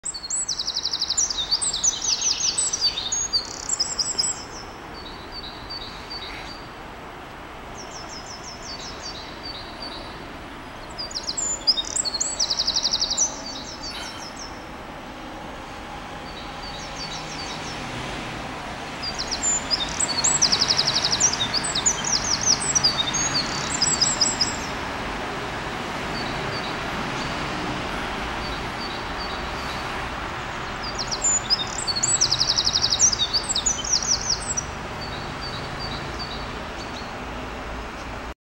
Eurasian wren (Troglodyte Mignon) in Paris
The 2nd smallest bird in Europe but sings like an opera singer.  This bird is not camera shy.  Taken in Alfred Capus park near la Porte d'Auteuil in Paris 75016.